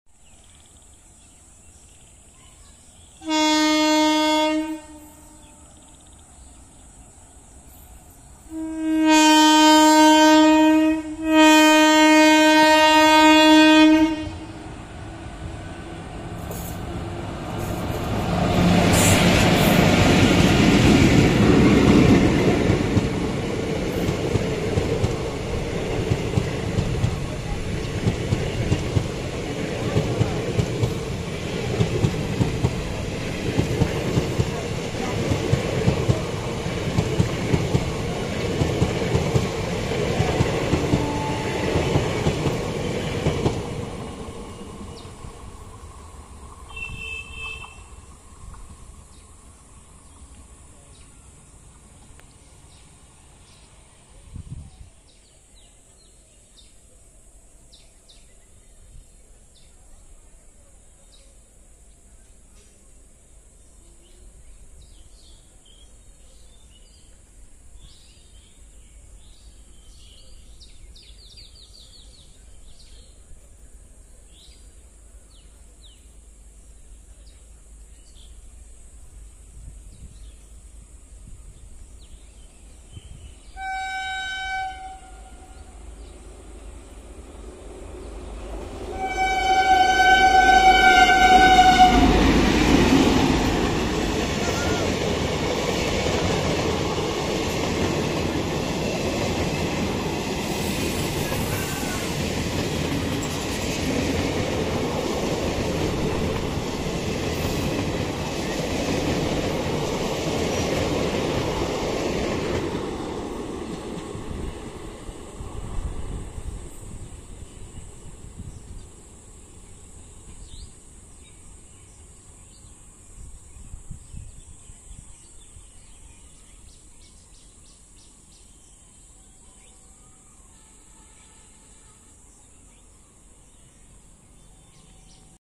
প্রবল ঝড়ের গতিতে আজিমগঞ্জ এক্সপ্রেস এবং জঙ্গিপুর এক্সপ্রেস বলাগর রেলগেট অতিক্রম করল
Double Engine WAP-4 powering AZIMGANG Express

HWH/WAP-4/22519
HWH/WAP-4/22521